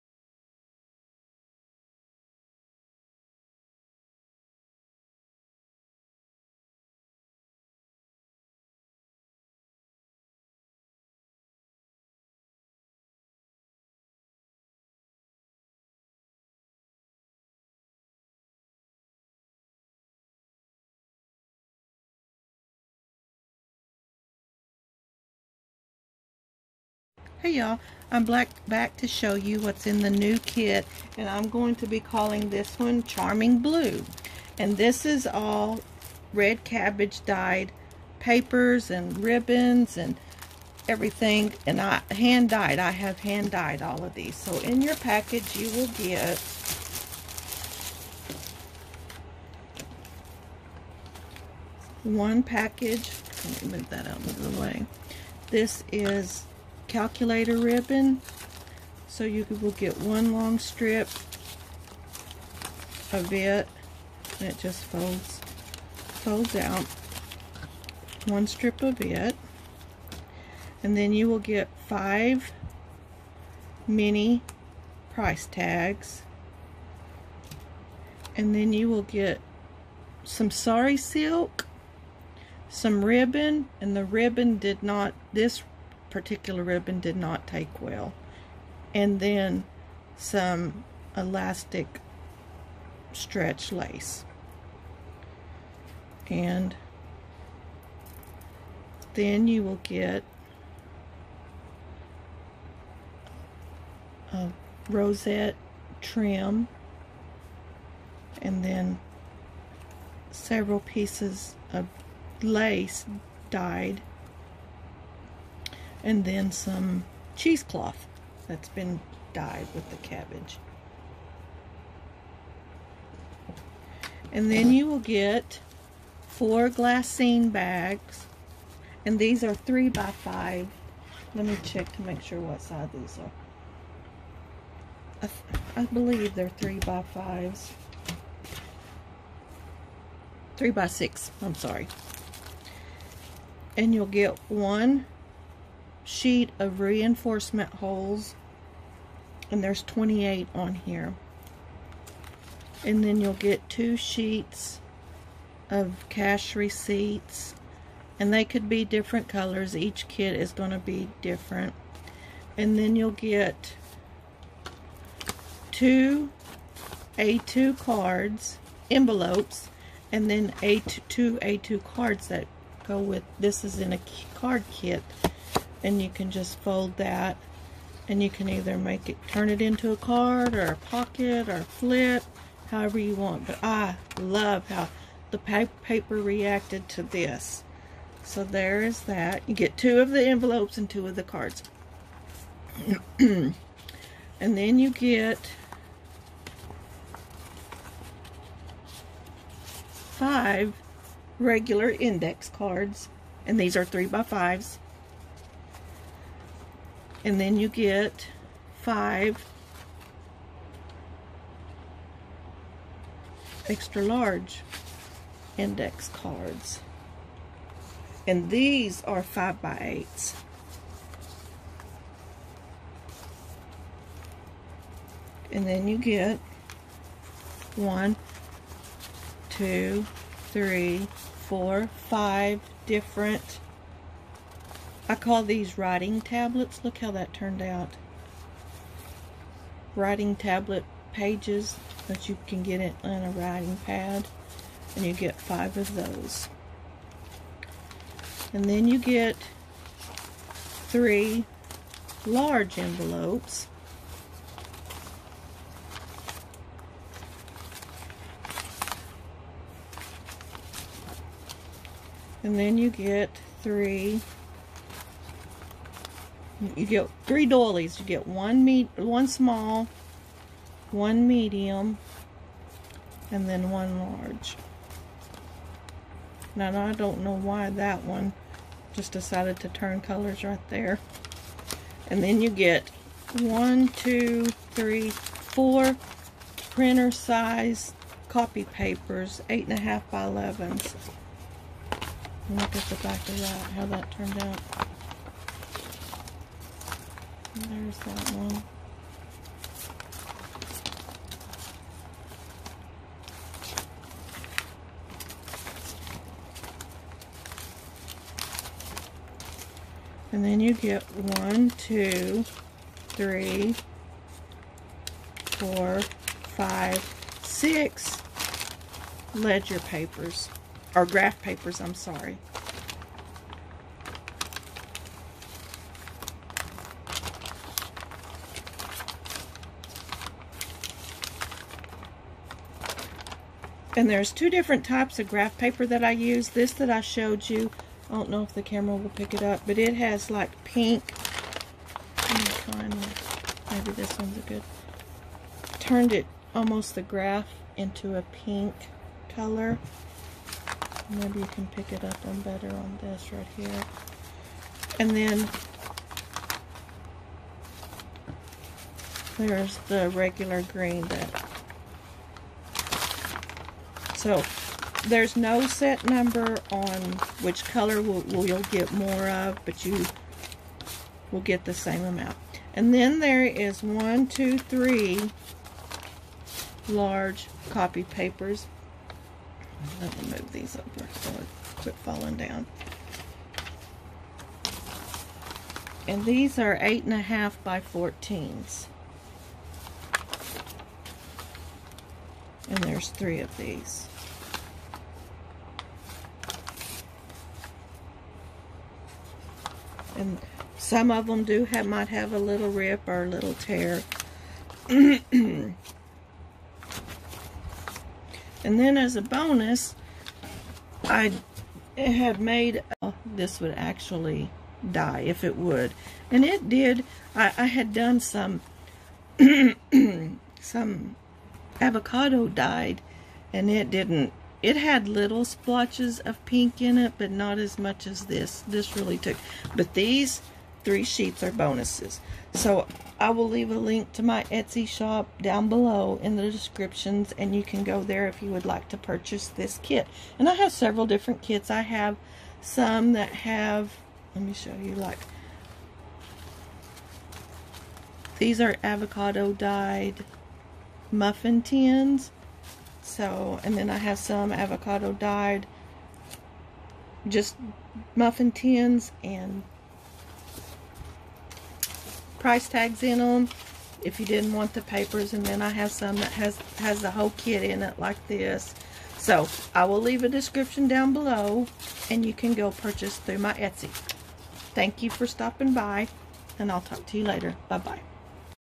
💙"Charming Blue" 💙 dyed papers for junk journal kits
"Charming Blue" red cabbage hand dyed by me. You get over 55 pieces of paper, trim, lace, fabric to decorate your junk journals with.

2 kits available in my Etsy shop.

Please share what you create. I would love to see it💖

My Etsy